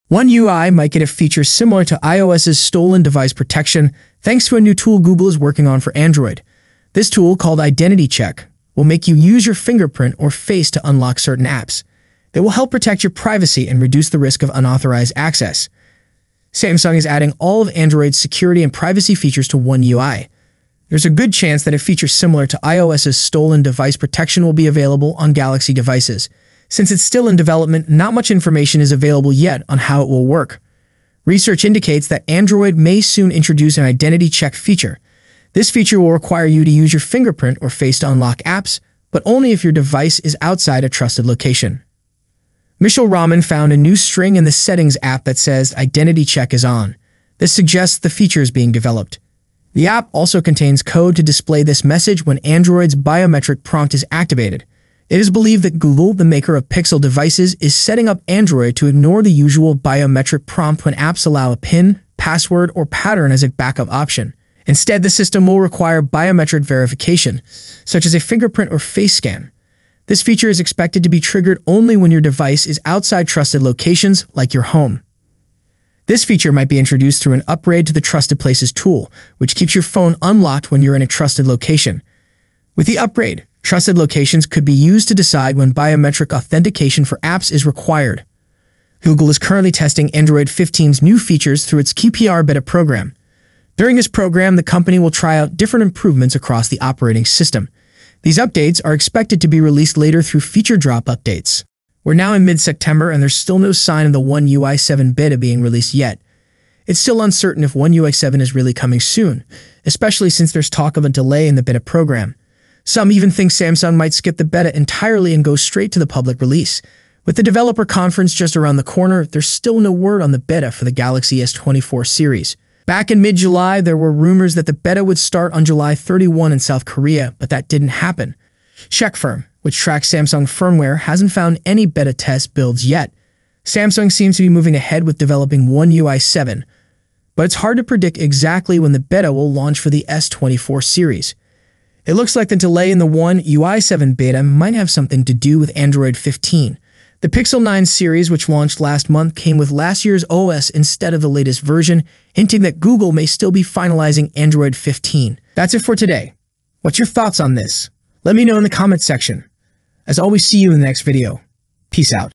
Samsung One UI 7 - HIDDEN NEW FEATURE REVEALED!!!
Samsung One UI 7.0 Android 15 - POWERFUL HIDDEN NEW FEATURE!
iOS's stolen device protection might be added to One UI on Android phones.

PEACE!!